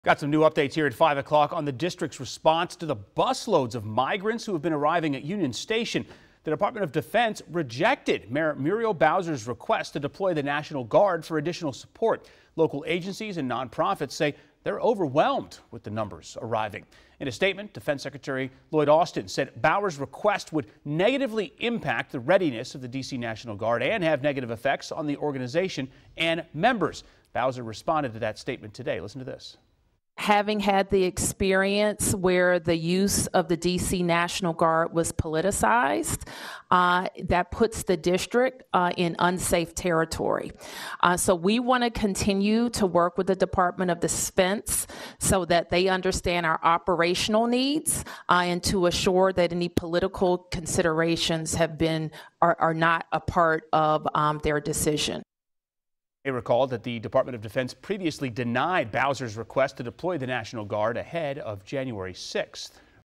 Pentagon denies DC request for National Guard migrant help
The Pentagon rejected a request from the District of Columbia seeking National Guard assistance in what the mayor has called a “growing humanitarian crisis” prompted